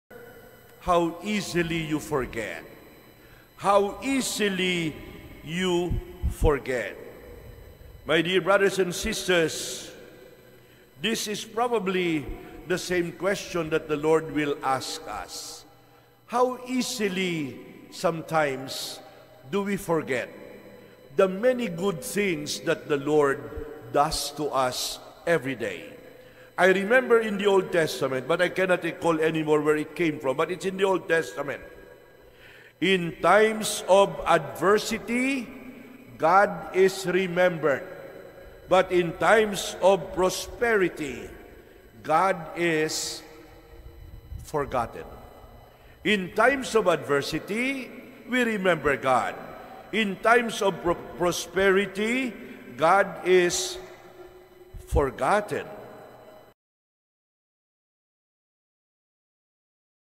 CLIP – HOMILY – REFLECTION OF REV. FR. LOUIE PUNZALAN, SVD
| ”…HOW EASILY YOU FORGET…” |

Holy Mass - 6:30am, Tuesday, February 13, 2024 at the Diocesan Shrine of Jesus the Divine Word of Christ the King Mission Seminary, Quezon City, Philippines.